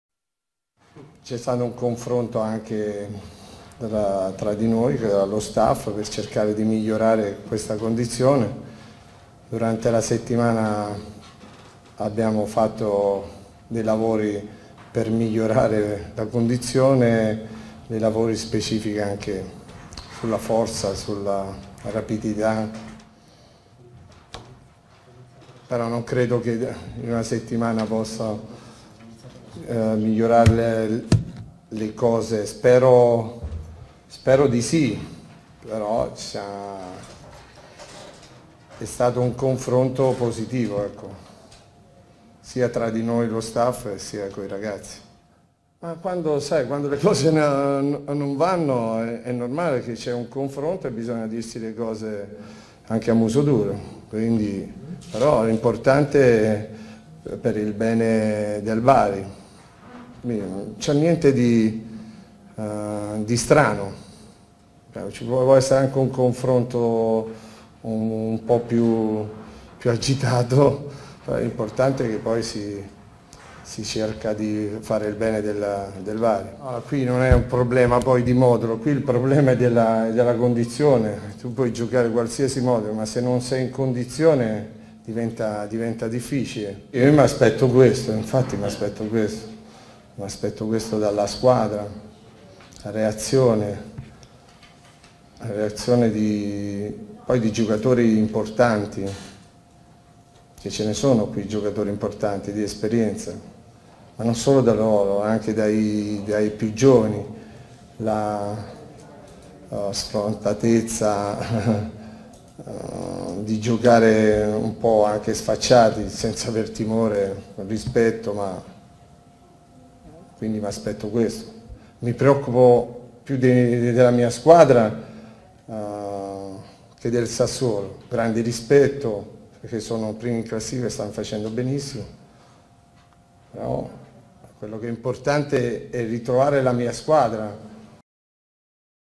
Intervista a Vincenzo Torrente
Intervista a Vincenzo Torrente(Allenatore del Bari), intervistato in sala stampa, alla vigilia della sfida di Serie.bwin contro il Sassuolo.